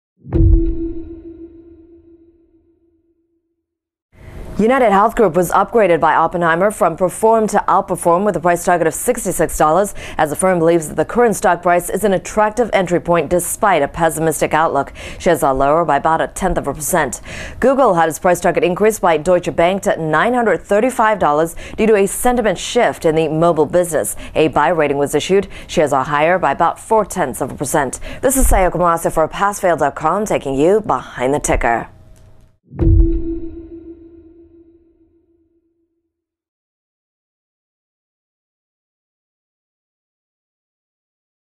Passfail.com News: Analyst Moves: UNH, GOOG
UnitedHealth Group (UNH) was upgraded by Oppenheimer (OPY) from perform to outperform with a price target of $66 as the firm believes that the the current stock price is an attractive entry point despite a pessimistic outlook.

Shares are lower by about a tenth of a percent.

Google (GOOG) had its price target increased by Deutsche Bank (DB) to $935 due to a sentiment shift in the mobile business.  A buy rating was issued.

Shares are higher by about four tenths of a percent.